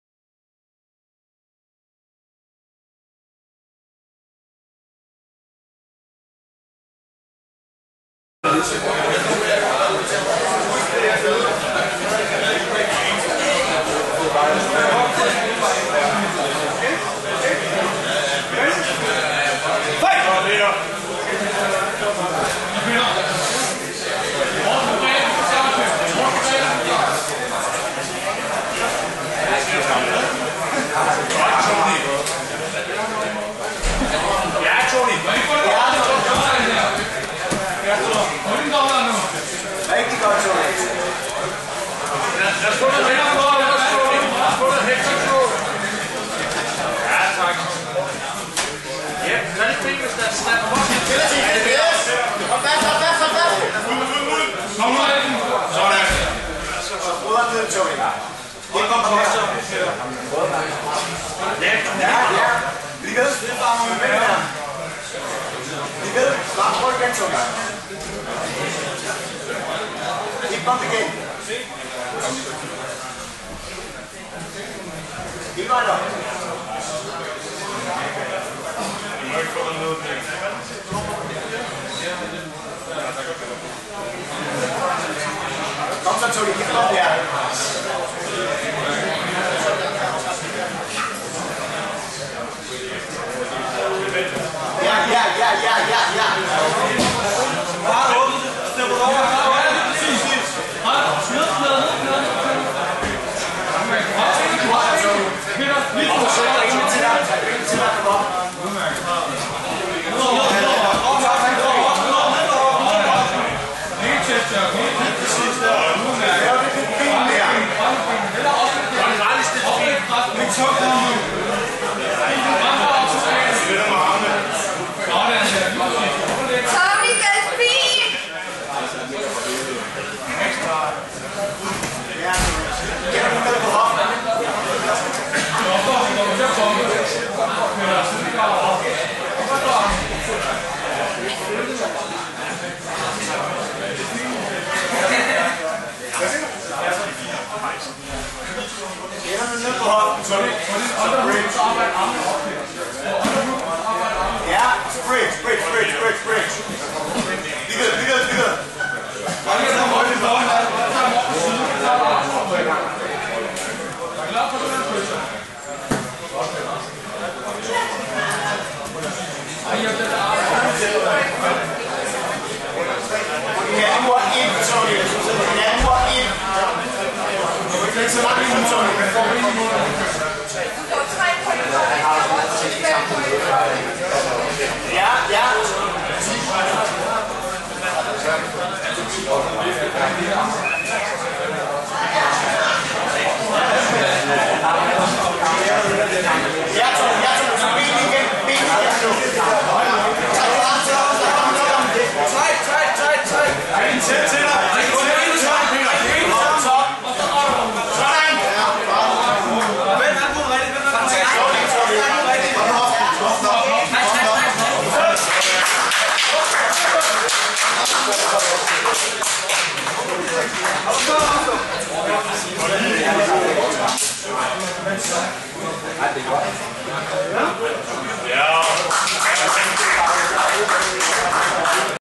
DGL Anden Runde DGL 2016 Tony Høy-Nielsen SIAM vs Peter Lassen Hillerød MMA og..
Dansk Grappling Liga 2016 Anden Runde DGL Sjælland
Group A  Øved Mænd -88.0 kg

Tony Høy-Nielsen SIAM been training 2 years 0 months vs Peter  Lassen Hillerød MMA og BJJ been training 1 years 7 months